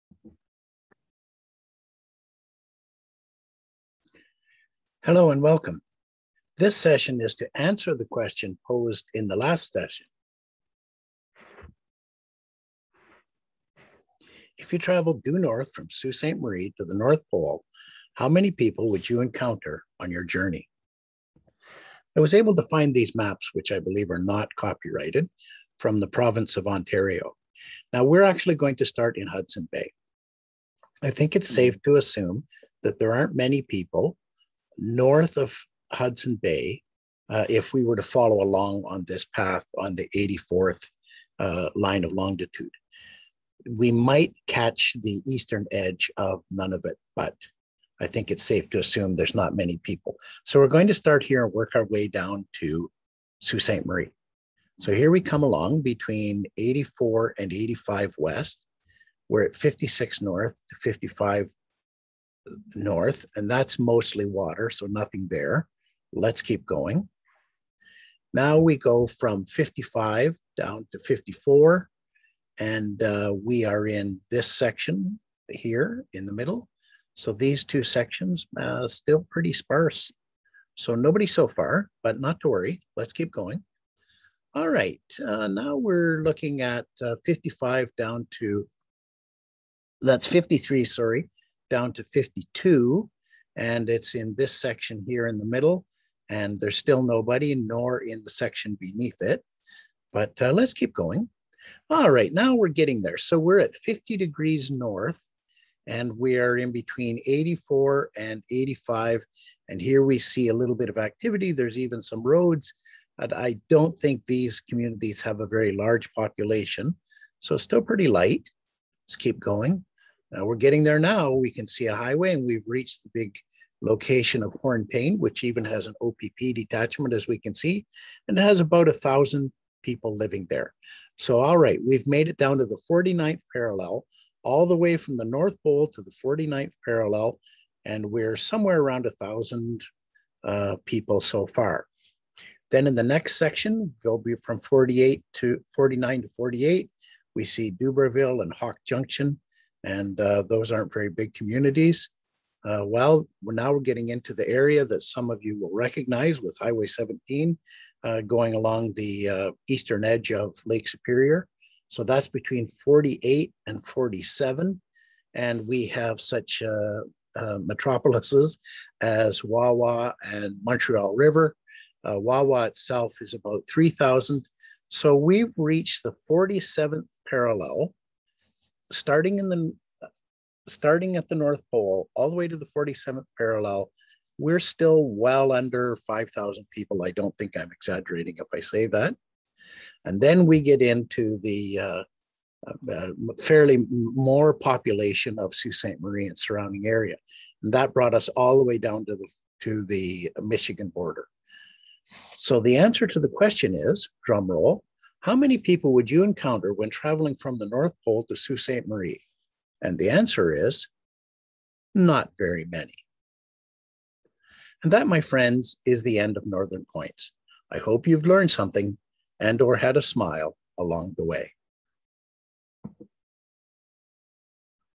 Chapter 20 of 20 How many people between Sault Ste. Marie and the north pole.
northernnotes Chapter 20 of 20 – Answer to the question, "How many people live between Sault Ste. Marie and the north pole?" (3 minutes 44 seconds)